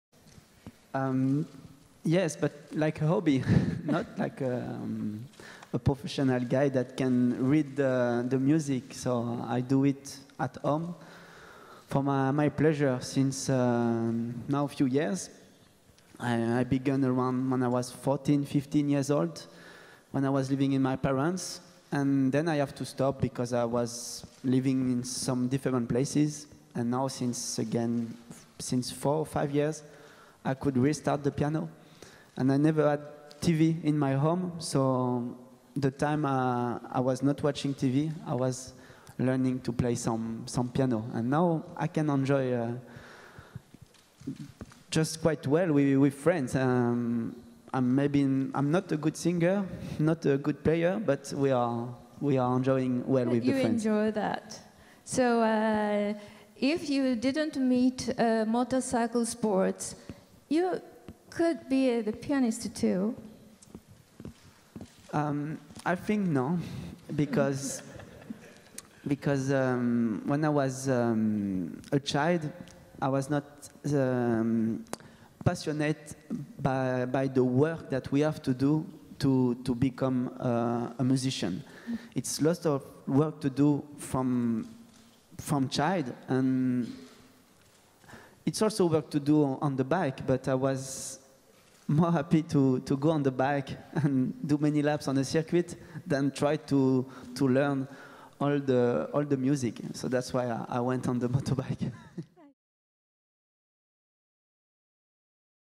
Johann Zarco's talk - "Two Yamahas, One Passion" RIDERS MEET PIANIST -
An unique collaboration between a pianist and motorcyclists thanks to Yamaha Motor Co., Ltd. and Yamaha Corporation.

ピアノ×バイク!?Yamahaだからできる異色のコラボレーション